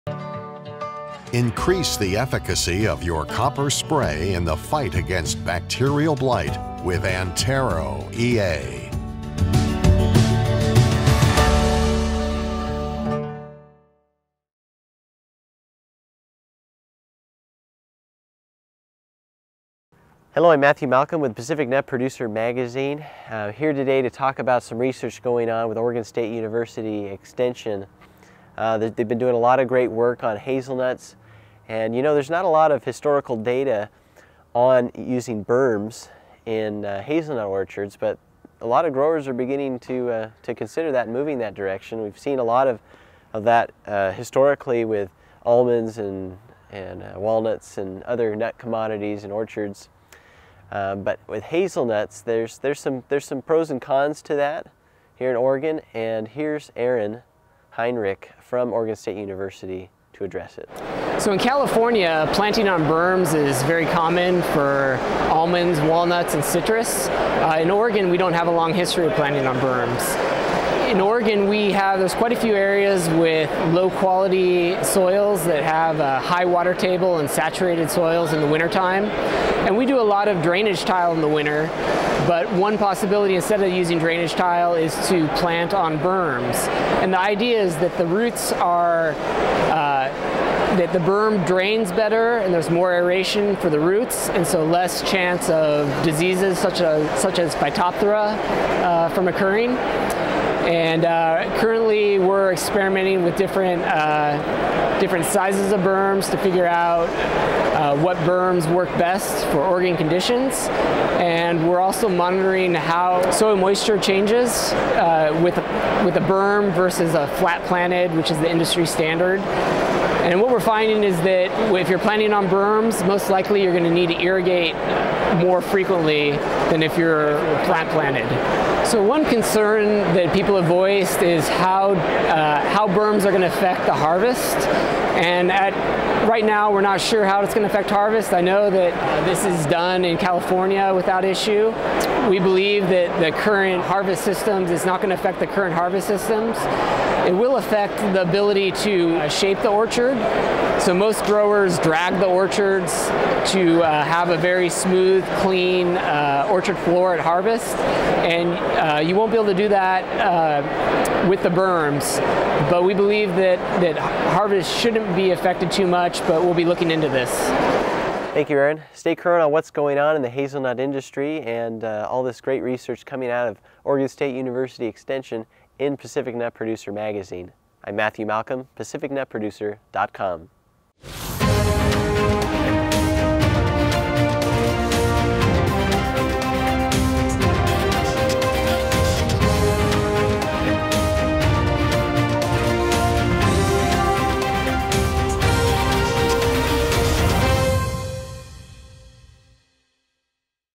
Pros & Cons of Planting Hazelnuts on Berms
Planting a new hazelnut orchard? Consider planting on berms. Check out this brief video interview with Aaron Heinrich from Oregon State University who shares some of the pros and cons of berms for hazelnut growers. Read more about growing hazelnuts in Pacific Nut Producer Magazine.  Don’t currently receive the magazine?